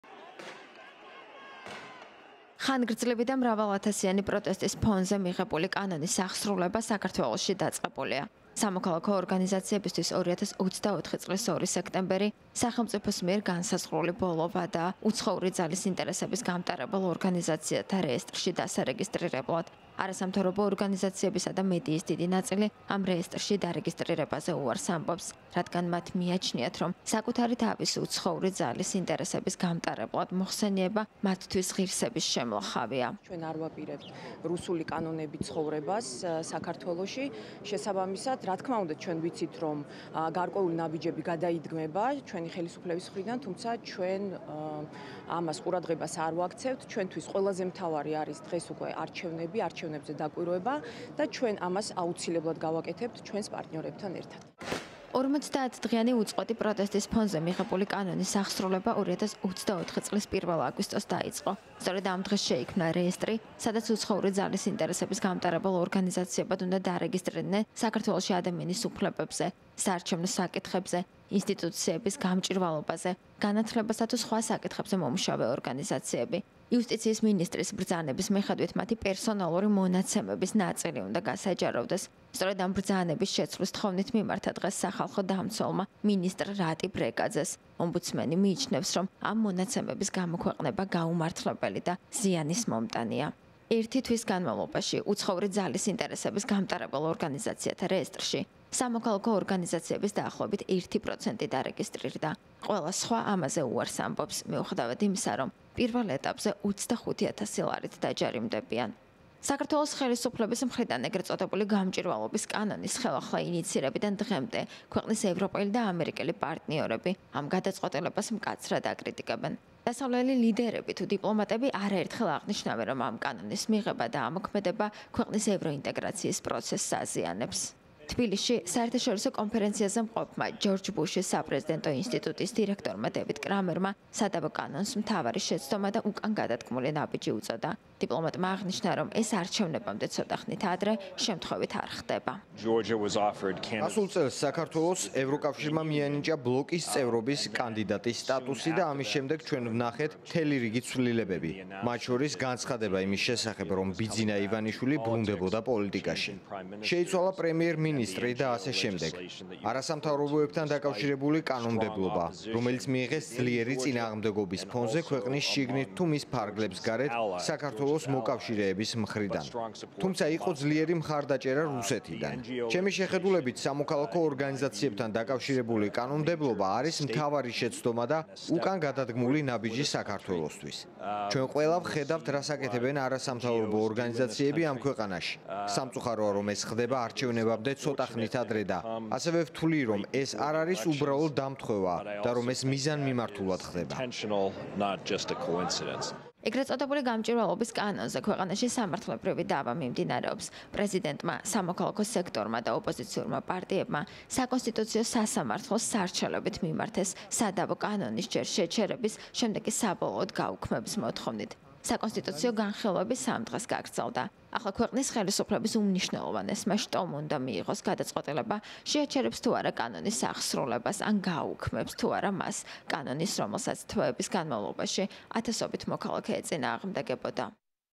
უცხოური ძალის ინტერესების გამტარებელ ორგანიზაციათა რეესტრში რეგისტრაციის ვადა ამოიწურა
„სამწუხაროა, რომ ეს ხდება არჩევნებამდე ცოტა ხნით ადრე. ვთვლი, რომ ეს არ არის უბრალო დამთხვევა და მიზანმიმართულად ხდება“, - განაცხადა ჯორჯ ბუშის საპრეზიდენტო ინსტიტუტის დირექტორმა დევიდ კრამერმა ე.წ. „გამჭვირვალობის“ კანონის შესახებ.